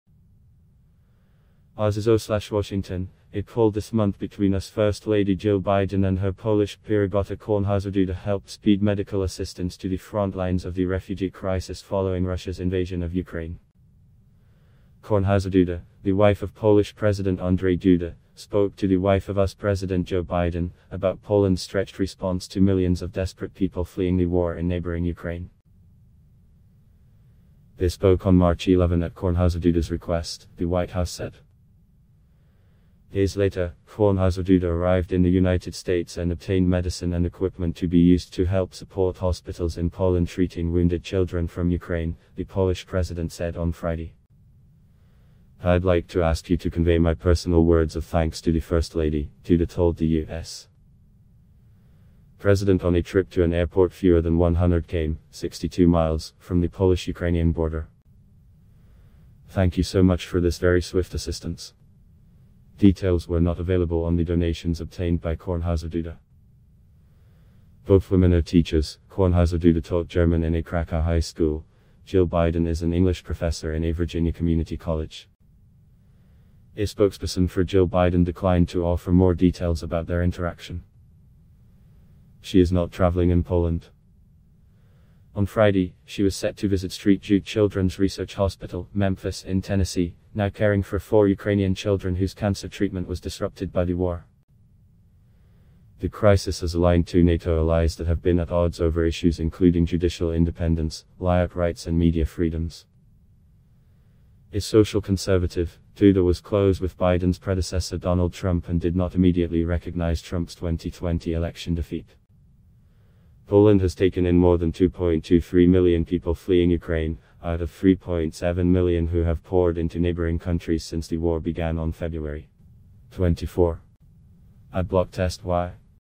US first lady, Poland president's wife partner on Ukraine aid
RZESZOW/WASHINGTON: A call this month between US First Lady Jill Biden and her Polish peer Agata Kornhauser-Duda helped speed medical assistance to the frontlines of the refugee crisis following Russia's invasion of Ukraine.